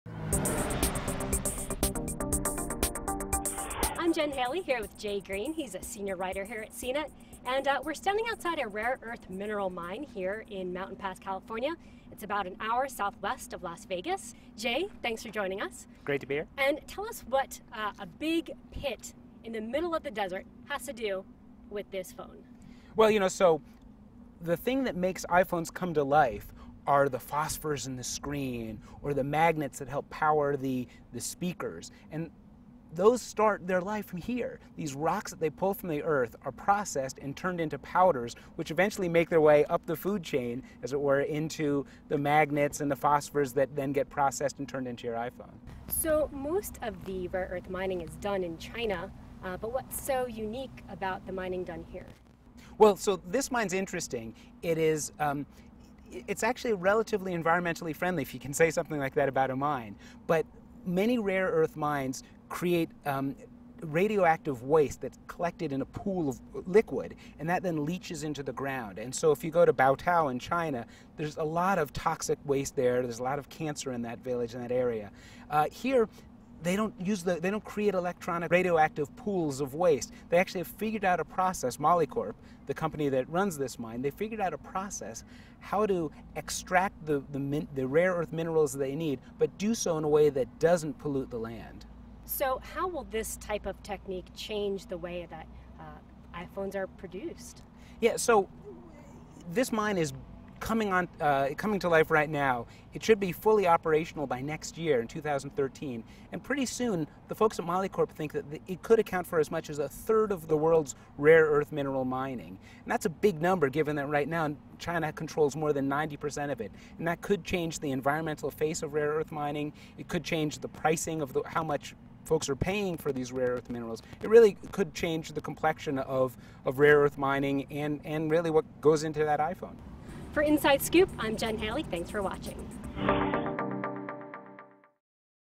CNET News - How rocks power your iPhone
CNET's Jen Haley talks to senior writer Jay Greene at the site of a rare-earth elements mine about how minerals become essential components in iPhones.